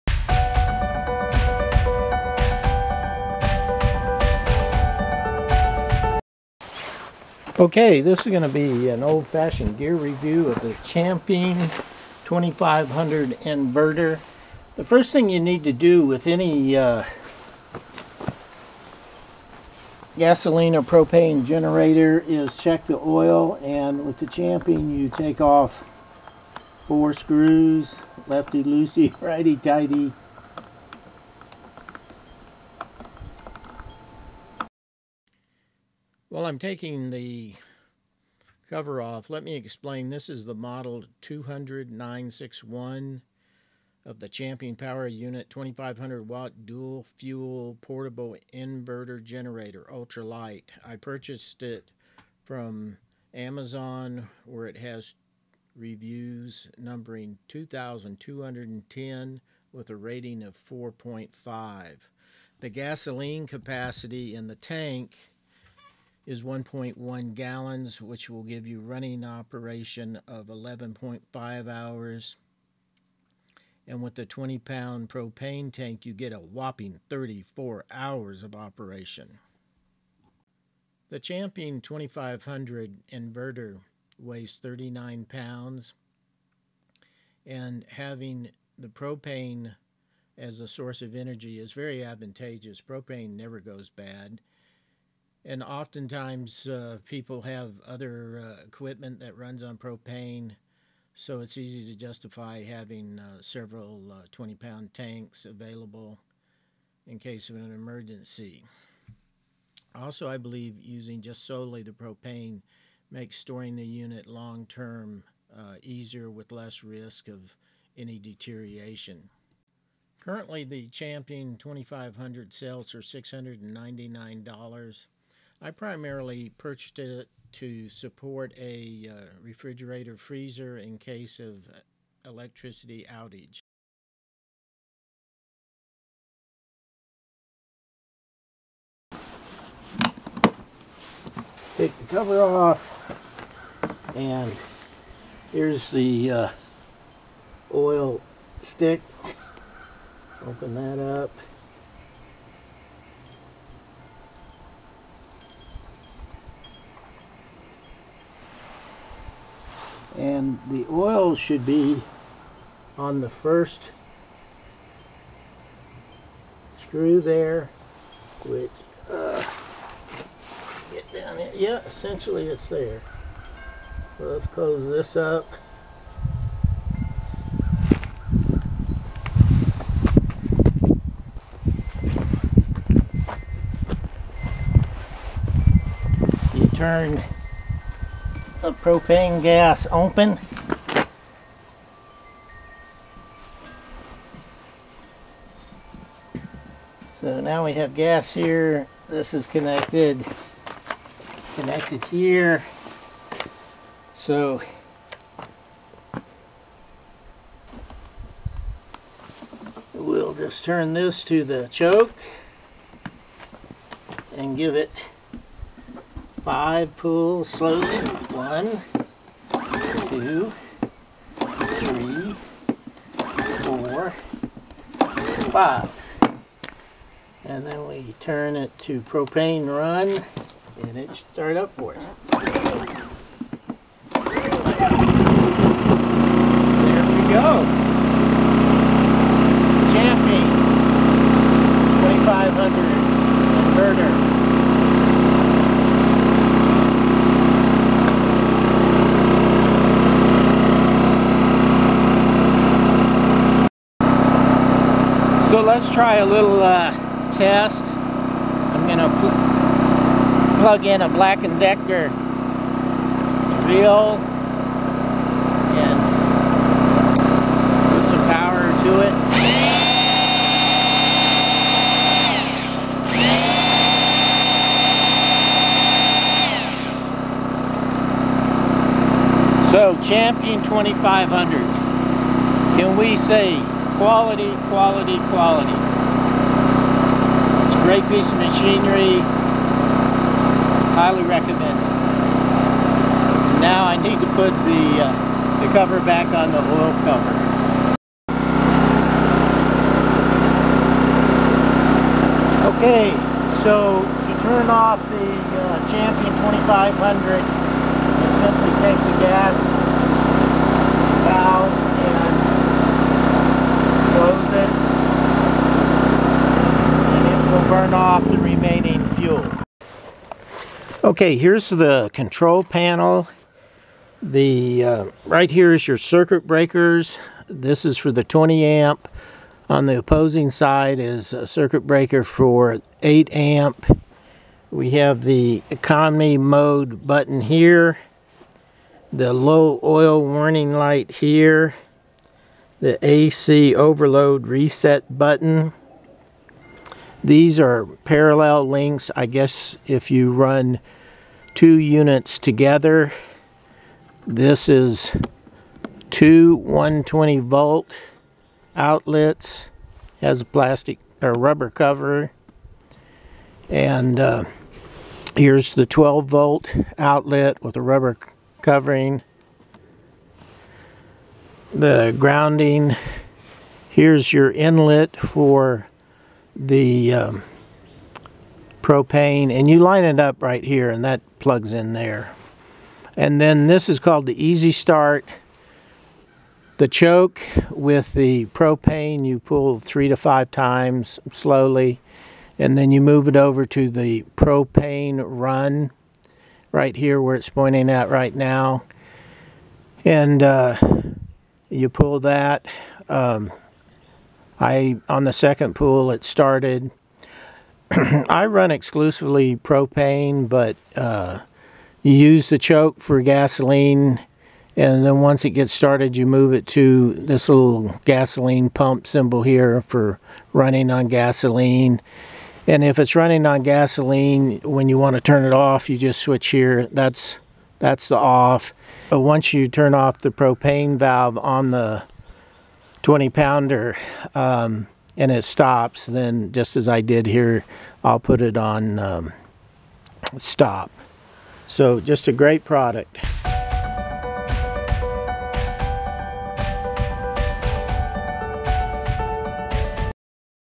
Champion 2500 watt Dual Fuel review with propane usage
Champion 2500 watt Dual Fuel portable inverter generator, ultralight model 200961 gear review.  Starting, demonstration with power tool, stopping, and explanation of control panel.